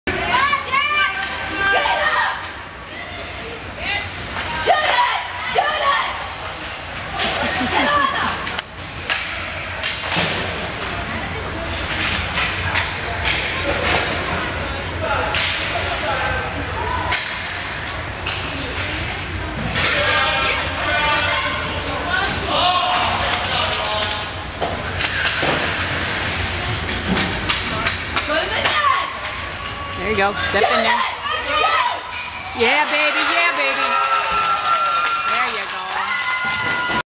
Jack Scores - Kissimmee Vipers vs. Glacial Sharpshooters MLK Tourney Squirt A
Jack with an assist to Adam.  Ultimate game winner.